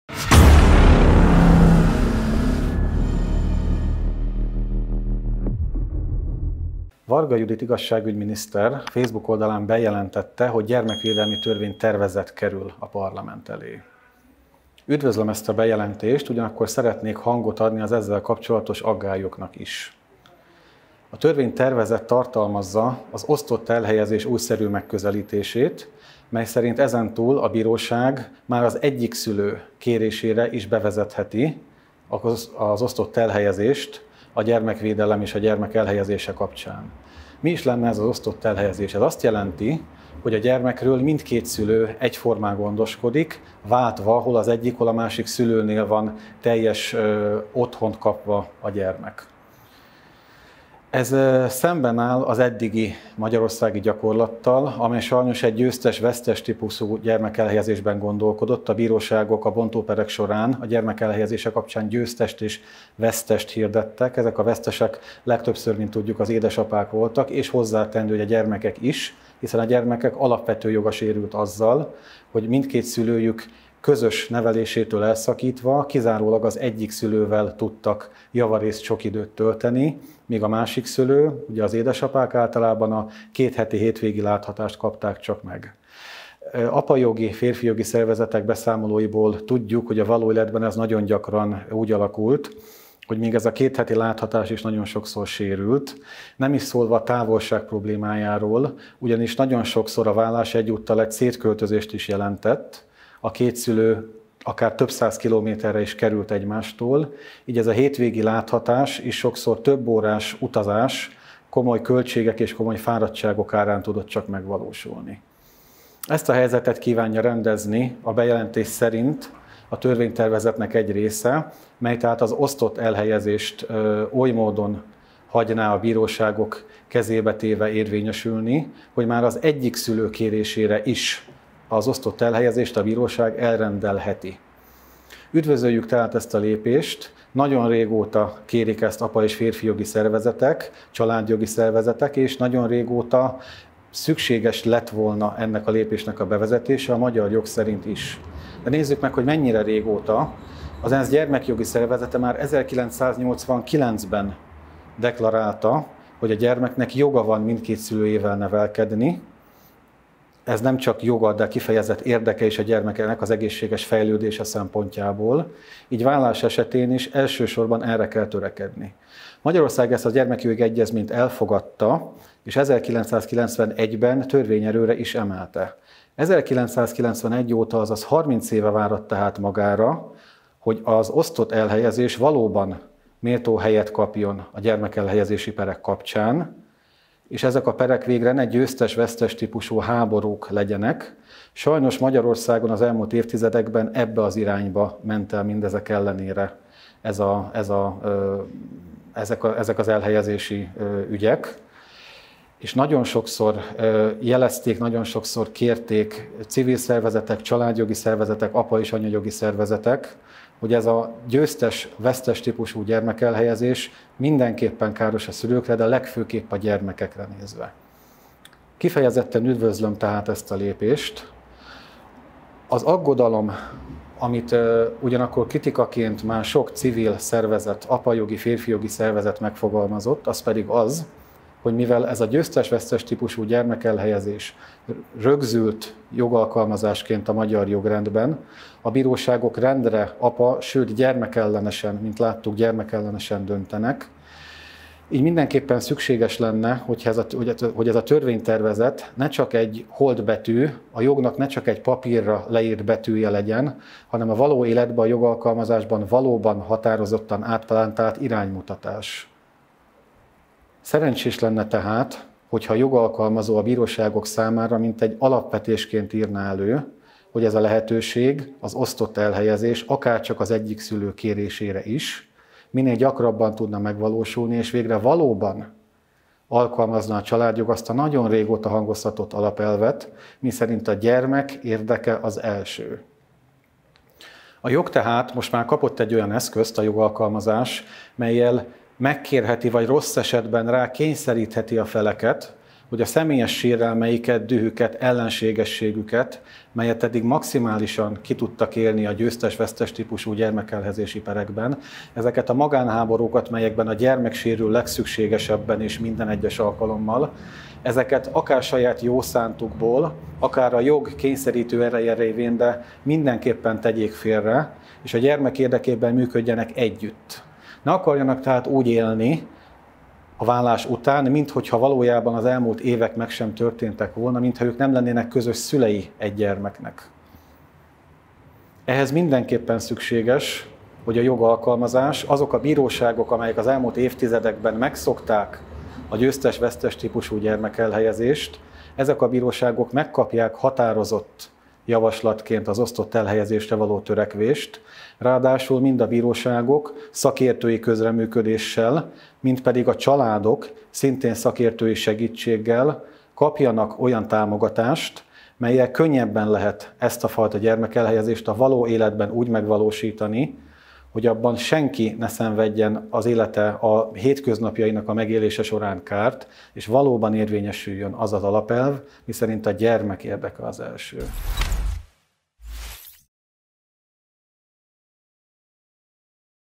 Az igazságos gyermekelhelyezésről
Halász Géza, a Mi Hazánk Mozgalom családjogi szakpolitikusának gondolatai az osztott elhelyezésről szóló törvénytervezet bejelentése kapcsán.